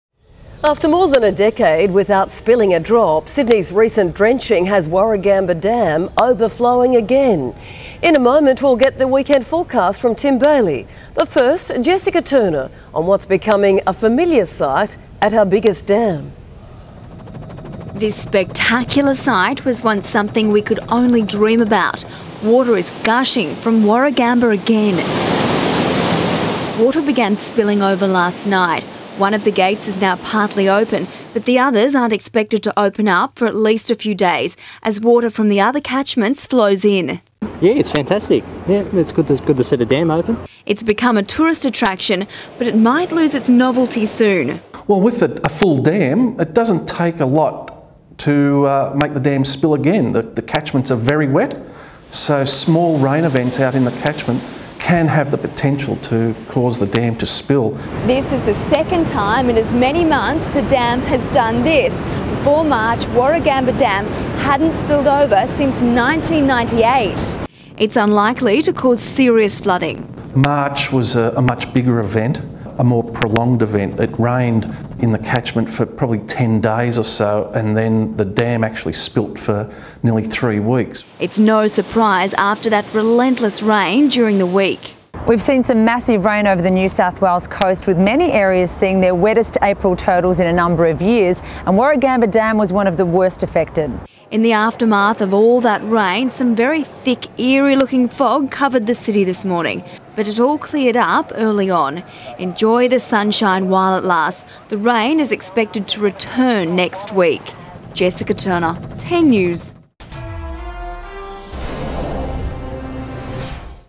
Rainfall windfall
After more than a decade without spilling a drop, Sydney's recent drenching has Warragamba overflowing again.